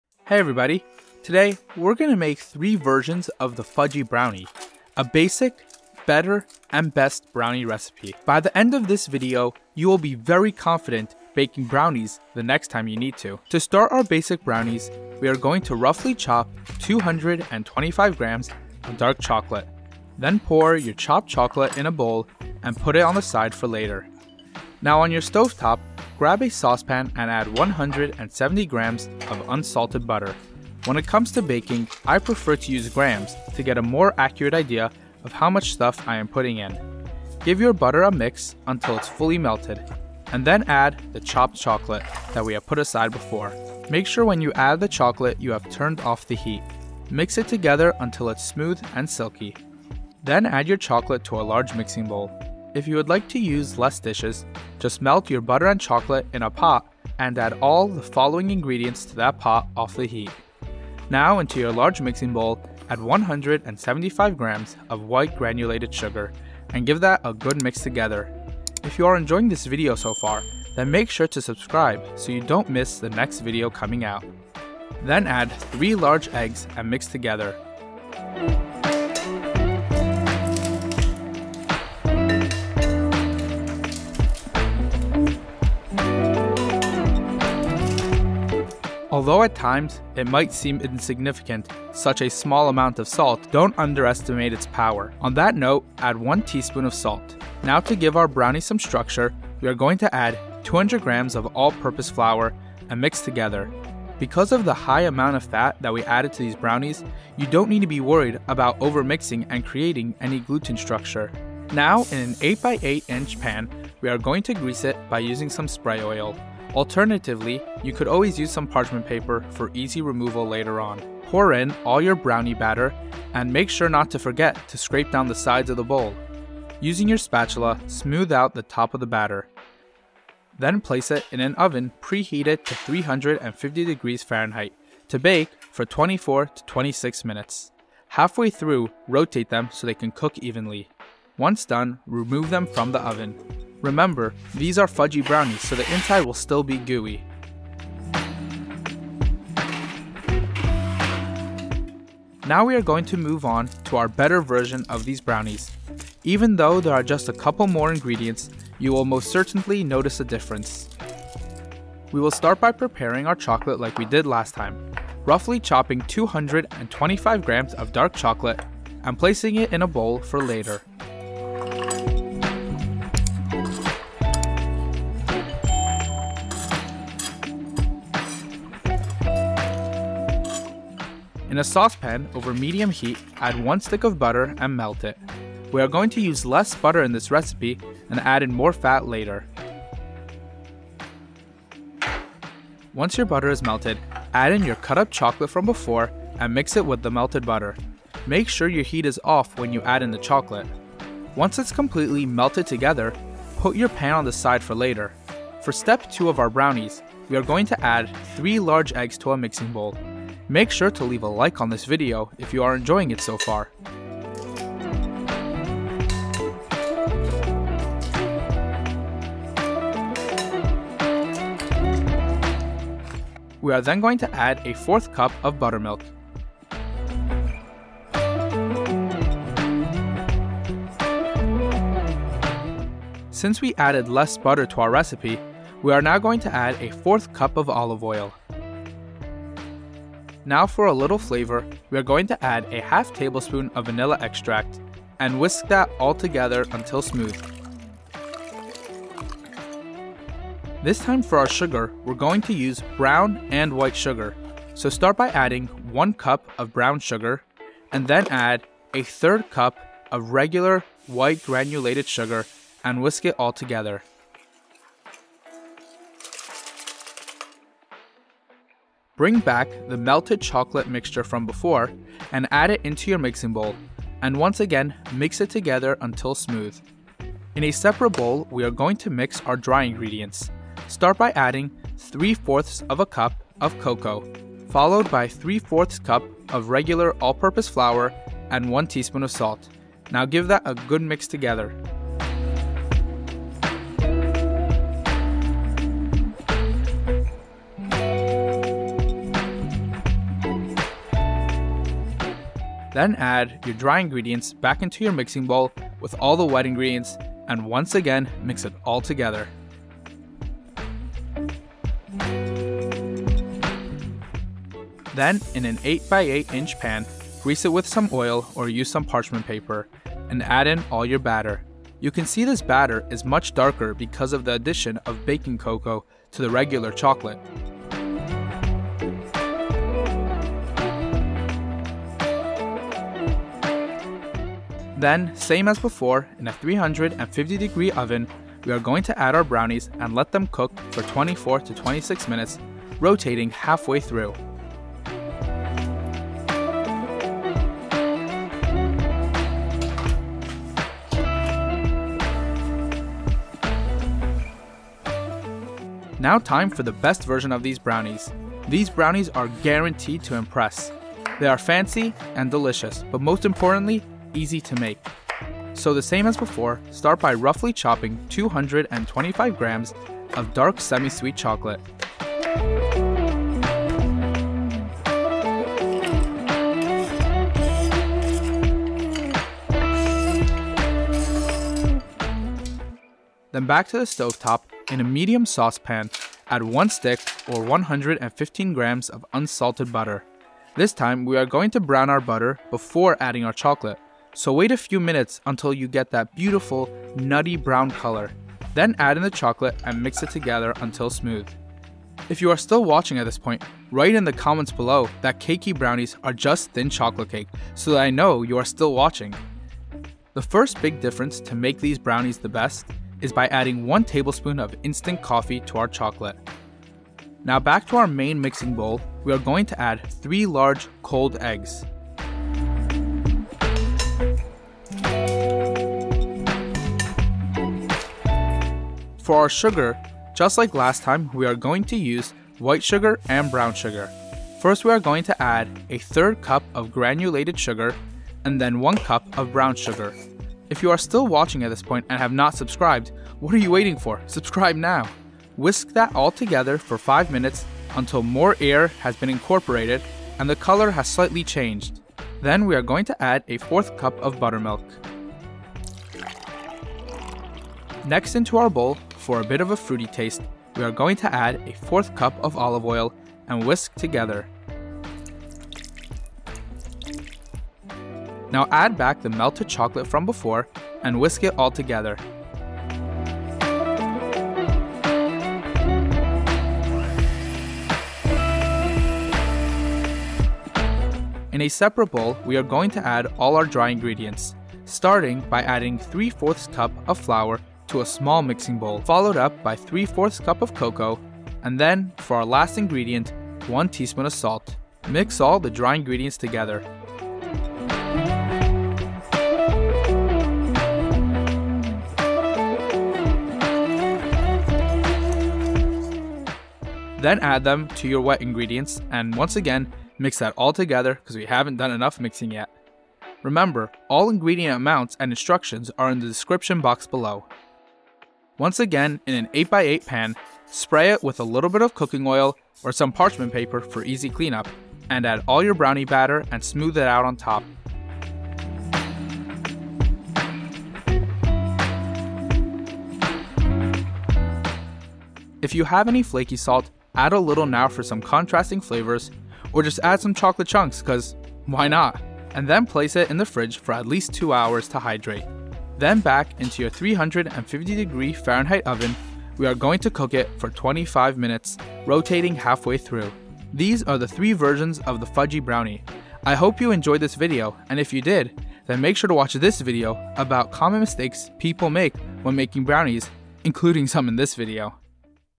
Fudgy Brownies 3 Ways
Are you looking for the perfect fudgy brownie? This Video will show you how to make the best fudgy brownie! Fudgy brownies are amazing but sometimes you don't know which ones to make. so this is the perfect fudgy brownie videos. Welcome to our kitchen! In this video, we're sharing 3 amazing fudgy brownie recipes that every chocolate lover needs to try. Whether you're searching for the best fudgy brownies with that perfect gooey texture or something with a crunchy twist, we’ve got you covered. These easy homemade brownie recipes are perfect for both beginners and seasoned bakers looking to create rich, decadent desserts.
If you're wondering how to make fudgy brownies from scratch, you’re in the right place. We’ll walk you through each step of the process, giving you tips on how to bake fudgy brownies with crispy edges and soft, moist centers. Whether you prefer classic brownies, double chocolate brownies, or brownies with nuts, these recipes are sure to impress!

Recipe:
Basic:
▪170g or 1.5 sticks unsalted butter
▪225g/8oz 70% chocolate
▪175g or 3/4c granulated sugar
▪5g or 1tsp kosher salt
▪3 eggs, whisked
▪200g or 1 1/4c flour

Melt butter on low in large sauce pot. Add chocolate and stir to melt. Add sugar & stir well. Off heat, add eggs and stir well. Add salt and flour. Stir until well combined. Load into a greased 8”x8”/20x20cm pan. Bake at 350F/175C for 24-26mi rotating halfway through.

BETTER
▪115g/1 stick butter
▪225g/8oz 70% cacao chocolate
▪3 eggs
▪50g or 1/4c buttermilk
▪60g or 1/4c olive oil
▪8g or 1/2Tbsp vanilla
▪100g or 1c brown sugar
▪75g or 1/3c granulated sugar
▪75g or 3/4c cocoa powder
▪100g or 3/4c flour

Melt butter on low in a large sauce pot. Stir in chocolate and remove from heat.

In a separate bowl, whisk together egg, buttermilk, oil, vanilla. Add sugars, whisk well. Add choco butter to sugar-egg mixture and stir.

In a separate bowl, whisk together flour, cocoa powder, and salt. Fold into wet mixture until well combined. Load into greased 8x8 or 20x20 and bake at 350F/175C for 24-26 min, rotating halfway through.

BEST
▪1 stick butter/115g
▪225g/8oz chocolate + extra for topping (or cacao nibs)
▪75g or 1/3c white sugar
▪3 eggs (cold)
▪5g/1Tbsp espresso/instant coffee

Brown the butter in a small saucepan preheated on high. When hot, add butter and stir constantly, scraping up solids at the bottom. When color is dark and golden, remove from heat. Off heat, stir in espresso and chocolate to melt.

In mixer, whisk eggs and sugars on medium high until thick and fluffy - takes several minutes. When done, the color should have lightened significantly. With mixer on low, add in oil, vanilla, buttermilk. Once incorporated, stream in chocolate-butter

In a separate bowl, whisk together flour, cocoa, salt. With mixer on low, add flour/cocoa mixture to wet ingredients. Use a spatula to scrape sides and to finish incorporating flour. Transfer mixture to prepared 8x8. Sprinkle with chopped chocolate chunks (or cacao). Cover and refrigerate for 2-24 hours to hydrate.

Sprinkle with flaky salt. Bake at 350F/175C for 24-26 min, rotating halfway through. Rest on counter to room temp, then refrigerate until cool.